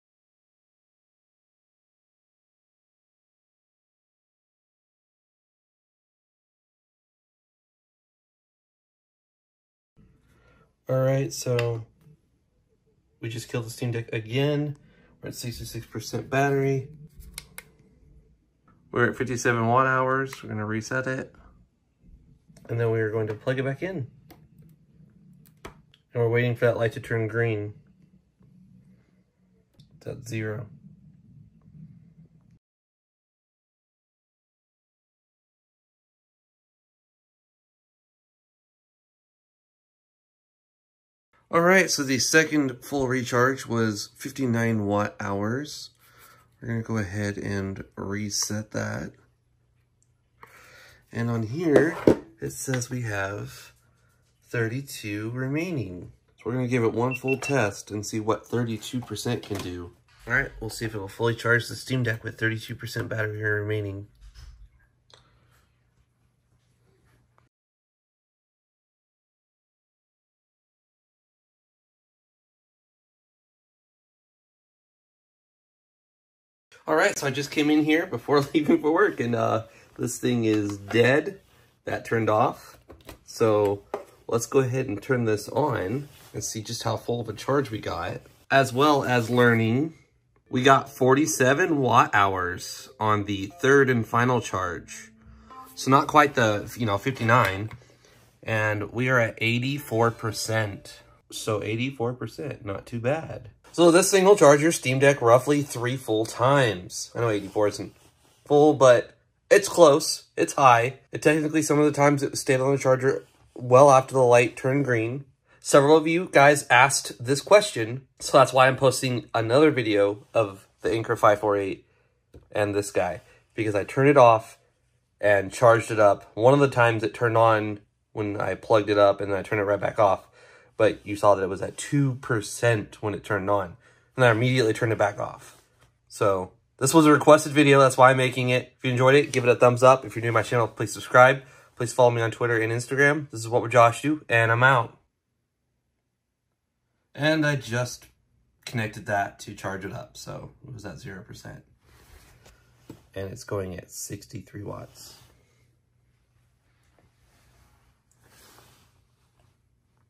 The Anker 548 will fully charge the Steam Deck 3 times!
Roughly 57 watt hours to fully recharge a Steam Deck that's at zero percent battery, to 100 percent battery. Ideally you don't take your steam deck below 10 percent, but I did... FOR SCIENCE!